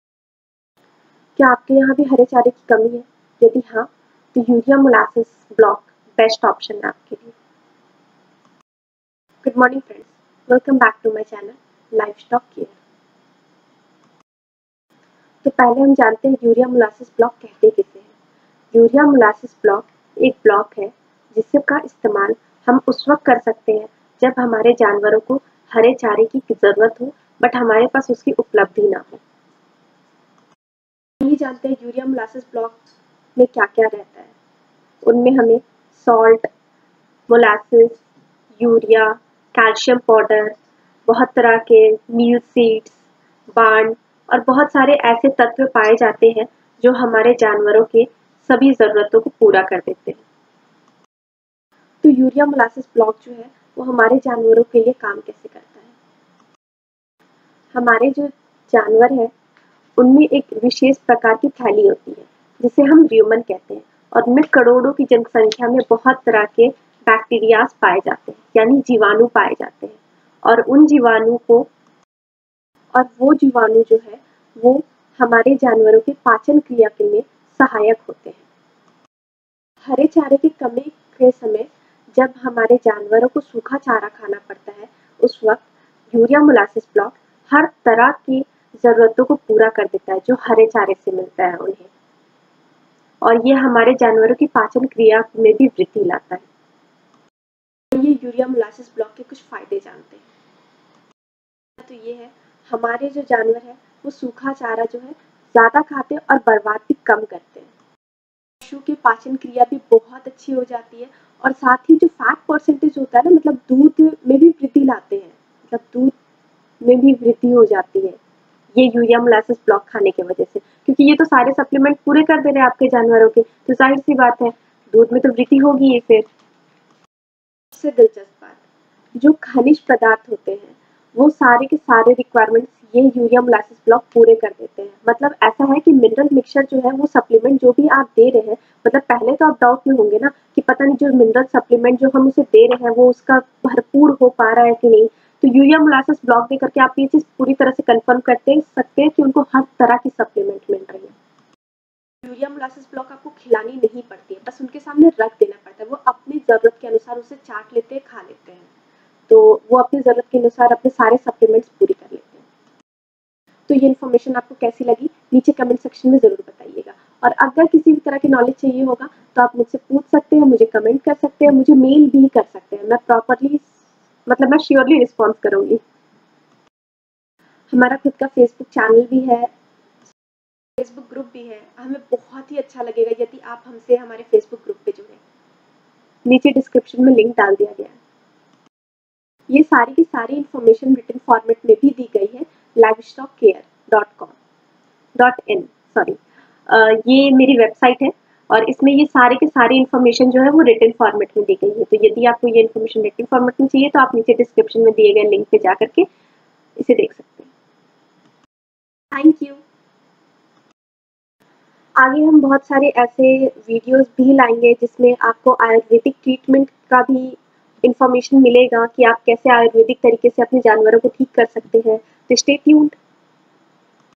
हरे चारे की कमी यूरिया मोलासेस है समाधान सिर्फ २०० ग्राम खिलाये और 2 लीटर तक दूध बढ़ाये
यूरिया मोलासेस ब्लॉक कहते किसे है ?
यूरिया मोलासेस ब्लॉक किन चीज़ो से बनता है ?
खनिज तत्व ,नमक , यूरिया , मोलासेस , मक्का ,सूखा चारा ,खली आदि चीज़े मिलती है
यूरिया मोलासेस ब्लॉक, हमारे जानवरो  को कैसे फयदा पहुँचता है ?